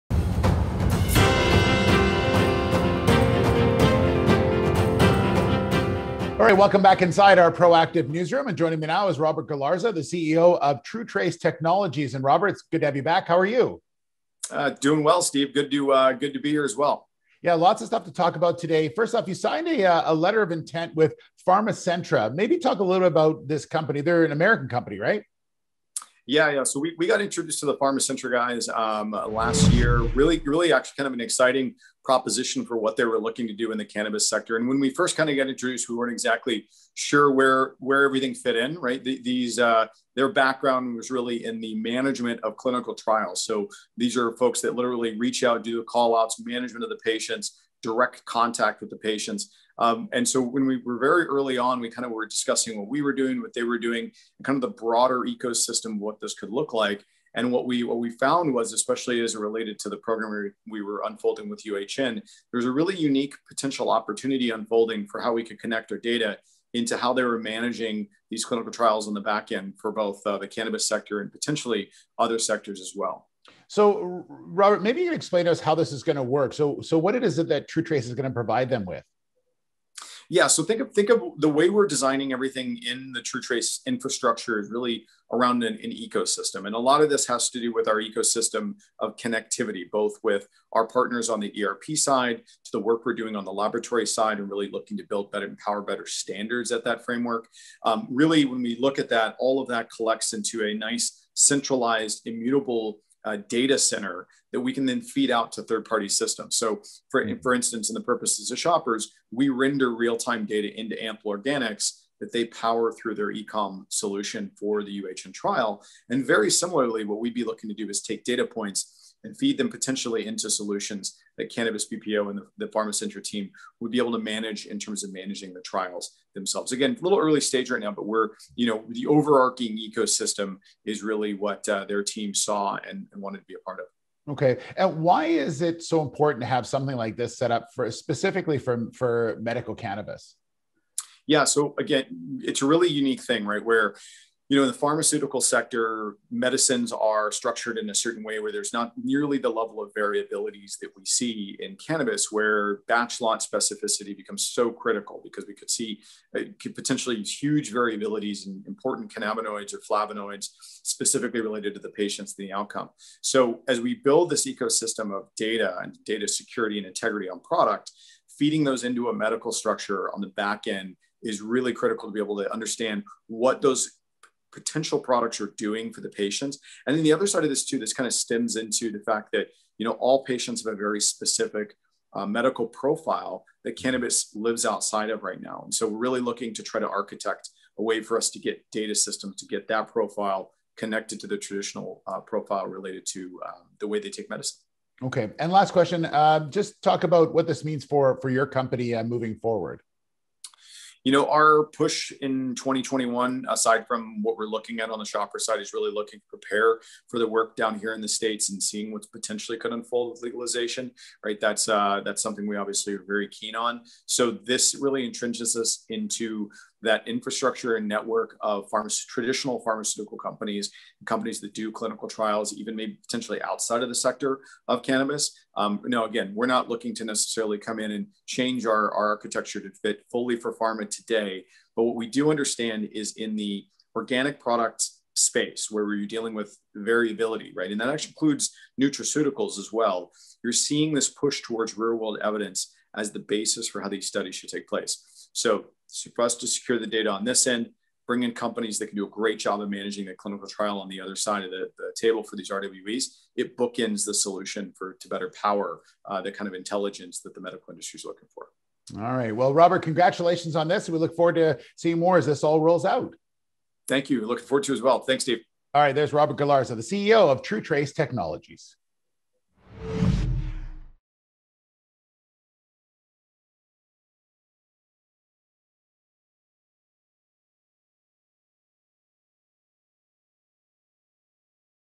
TruTrace Technologies signs LOI to provide blockchain-secured ecosystem for medical cannabis
TruTrace Technologies (CSE: TTT- OTCQB: TTTSF) CEO Robert Galarza joined Steve Darling from Proactive to bring news the company has announced that it has signed a letter of intent with Georgia-based PharmaCentra to support a clinical trial ecosystem built on standardized protocols and testing of cannabis products for use in the medical industry.

Galarza sharing with Proactive this is a very important step for cannabis, which is a useful component of therapies to treat serious conditions because it ensures all products will be safe and consistent.